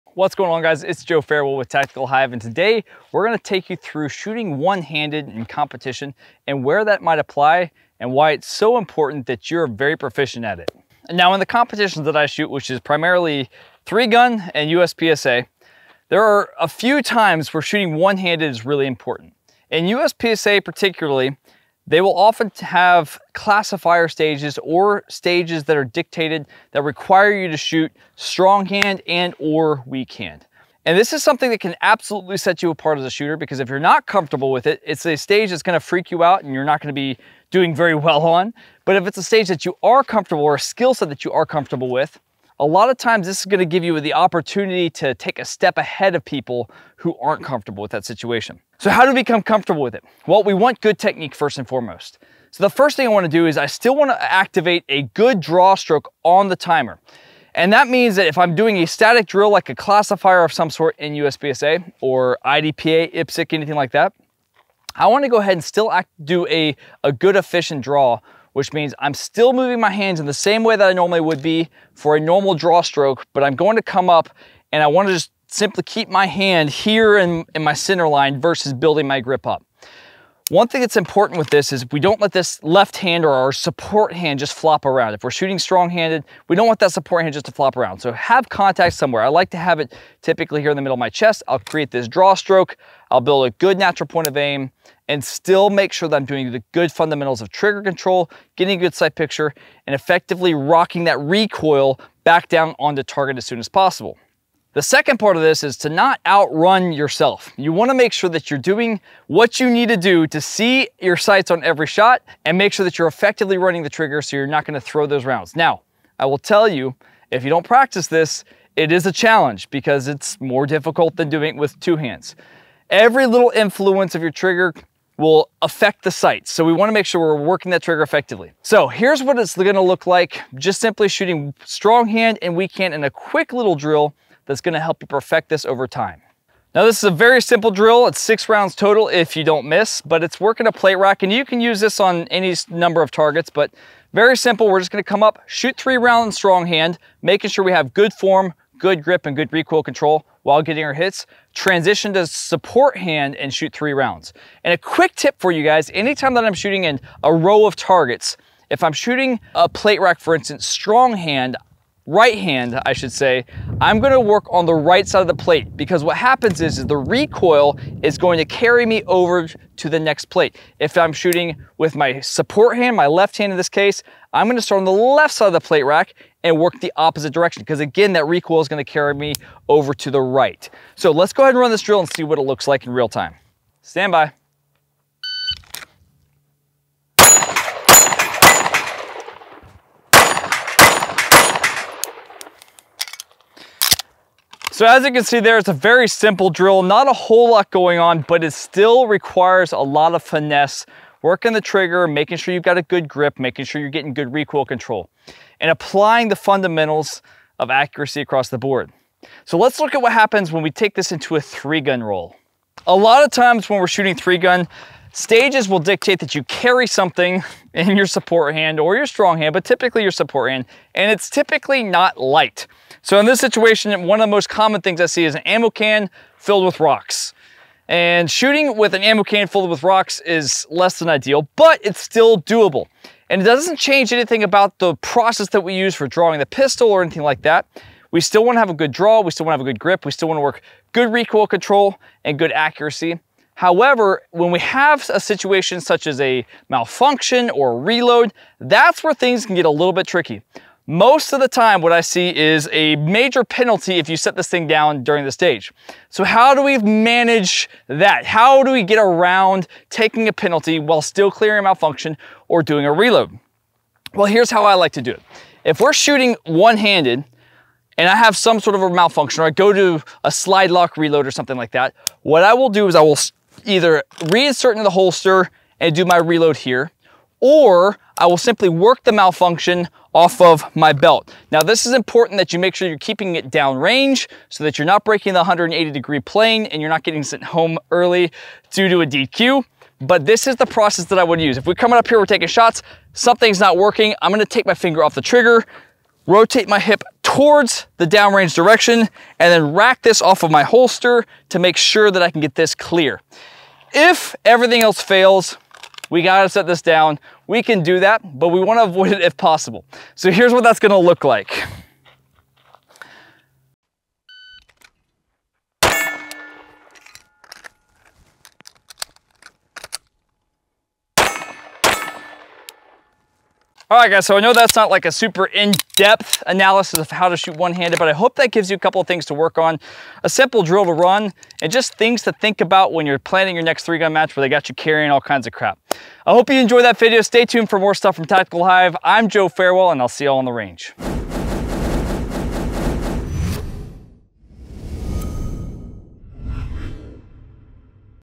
How To Shoot A Pistol With One Hand
Former SWAT member and 3-Gun National Champion, Joe Farewell, demonstrates how to shoot a pistol with one hand.

If you participate in competition shooting, you will have to shoot one-handed at some point, so learning how to shoot with one hand and practicing one-armed shooting is a must.

Shooting with one hand is critical for self-defense as well, as evidenced by the numerous footage of real-life incidents.